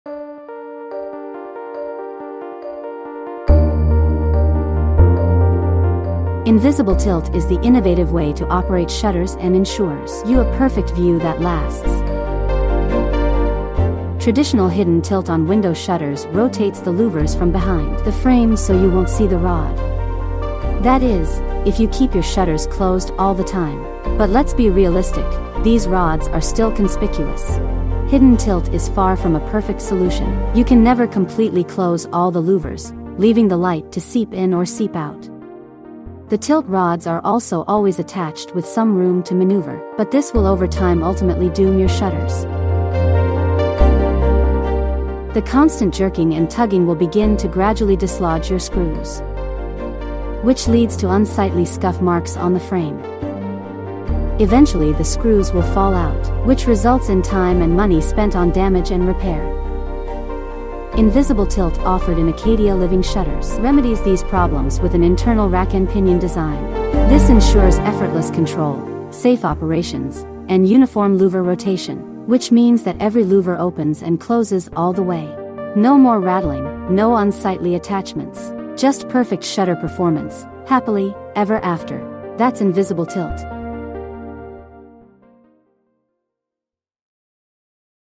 Acadia Living - Invisible Tilt®
TiltFree® shutters guarantee crystal-clear views and add a streamlined, sophisticated look to any room. Learn more about Acadia Living’s exclusive feature in this video.